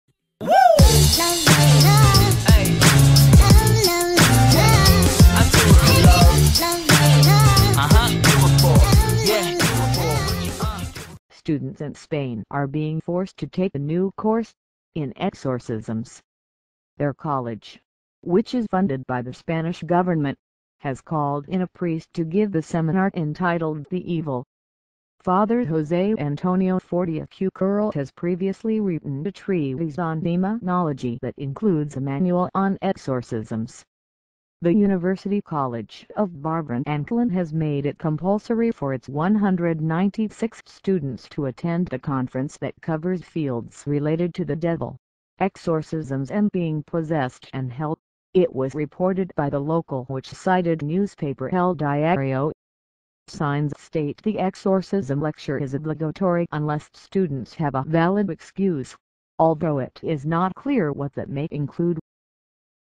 Exorcism lessons are made compulsory for Spanish students – with a seminar entitled 'The Evil'
Students in Spain are being forced to take a new course – in exorcisms.
Their college, which is funded by the Spanish government, has called in a priest to give a seminar entitled 'The Evil'.
Father José Antonio Fortea Cucurull has previously written a treatise on demonology that includes a manual on exorcisms.
The University College of Barberan and Collan has made it compulsory for its 196 students to attend the conference that covers 'fields related to the devil, exorcisms and being possessed and hell', it was reported by The Local which cited newspaper El Diario.
Signs state the exorcism lecture is 'obligatory' unless students have a valid excuse, although it is not clear what that may include.